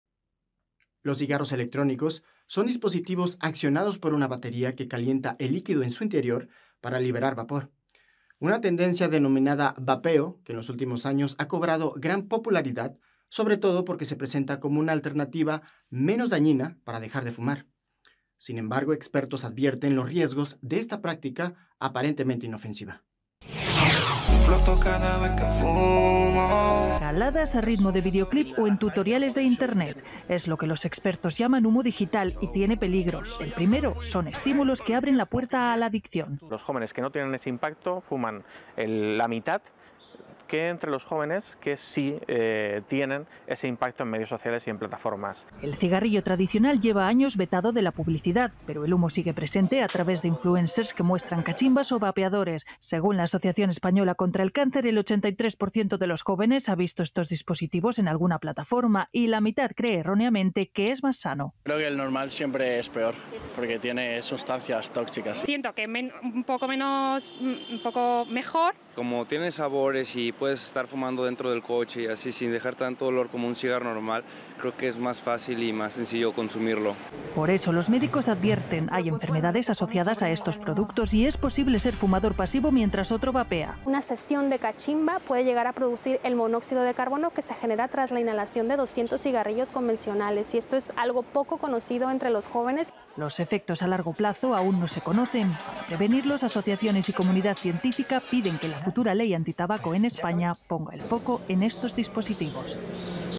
La adicción a los vapeadores aumenta entre los jóvenes
Expertos alertan sobre la difusión sin relugación que existe sobre el uso de los "vapes", pues al menos en España, los jóvenes creen erróneamente que fumar desde un vapeador es más sano que fumar tabaco normal.

Foto de portada: Pixabay.